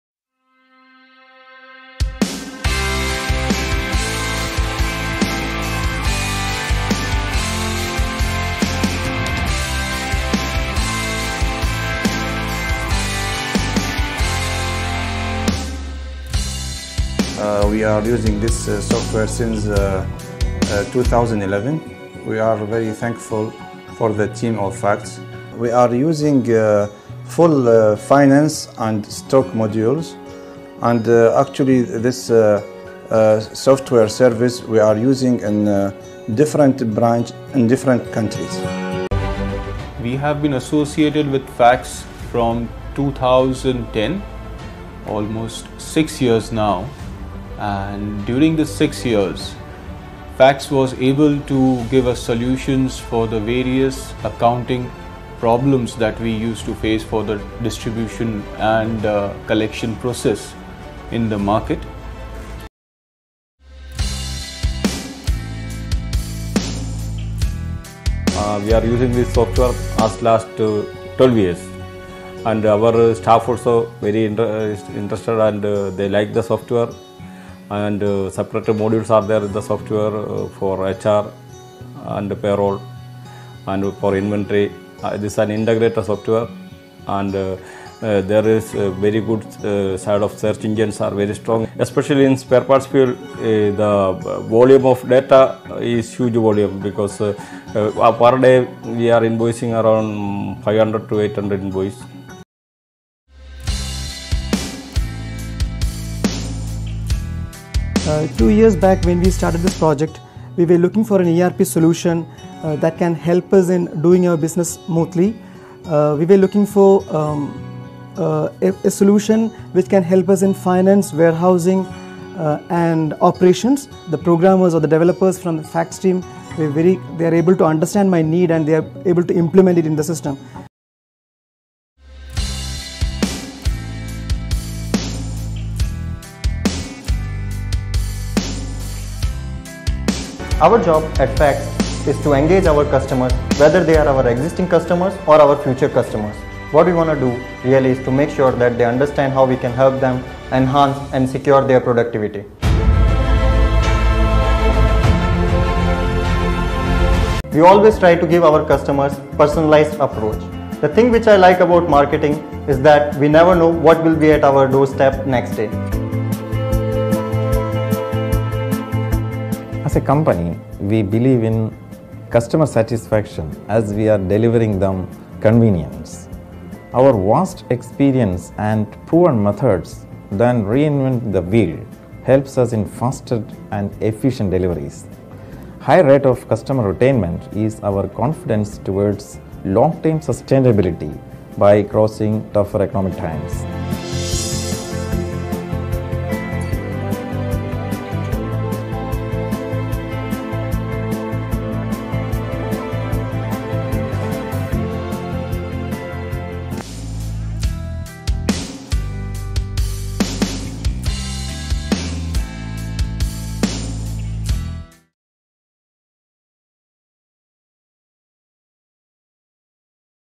Why FACTS-ERP? Hear From Our Customers!
Here's some of our loyal customers sharing their experience with FACTS ERP, our customizable ERP software.